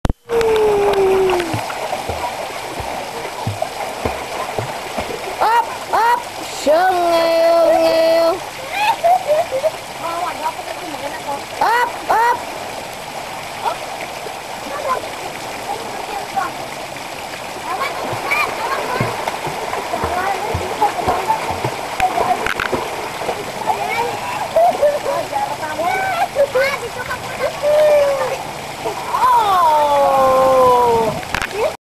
Blue Jazz pool-Samal-3 (09/15/10)
1 year en 6 months old baby wanted already to swim alone in de pool.